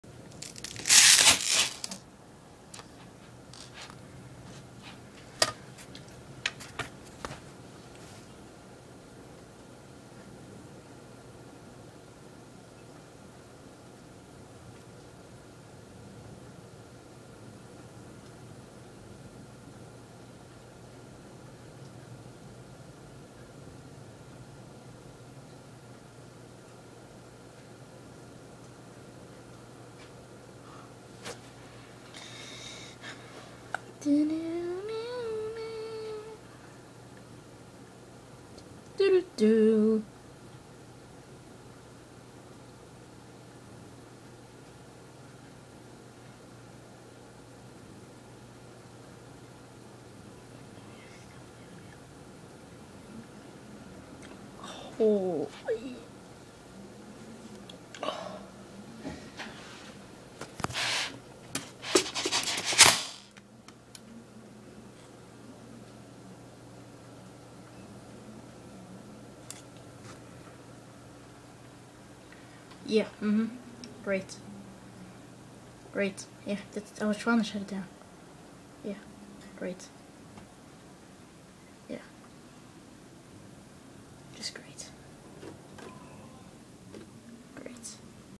Blue screen fail
Ovisuly windows vista can't do the simpelest task